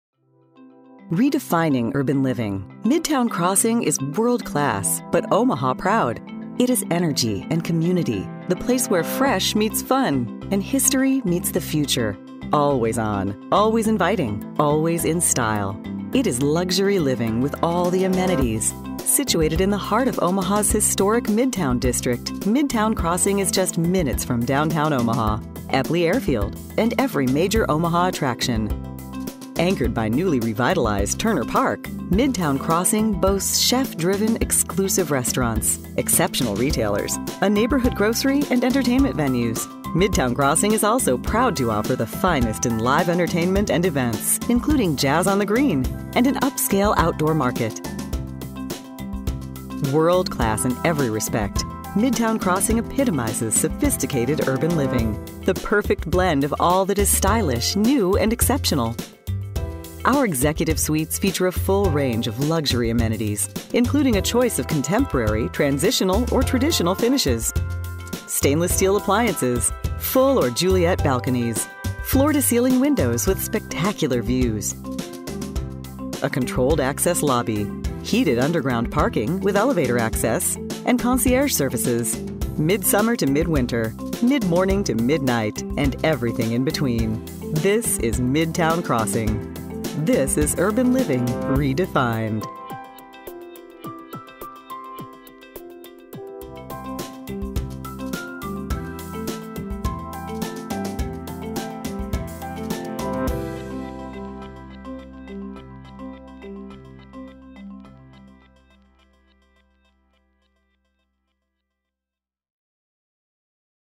Midtown Crossing Executive Suites
Video production for Lund and the Midtown Crossing Executive Suites.  Shot and edited by Delinea Design.